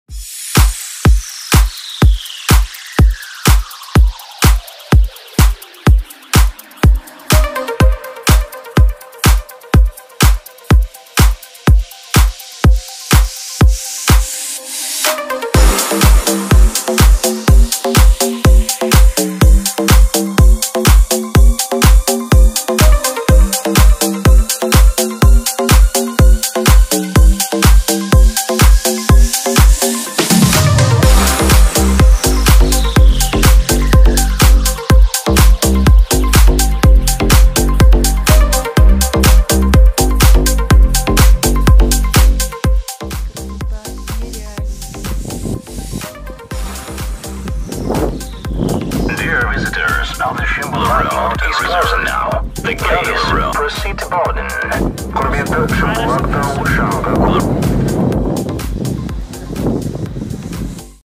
Канатка на Чимбулак
Я полюбила горы, но ещё не до конца. Потому что не прочувствовала все прелести связанные с ними в полном объёме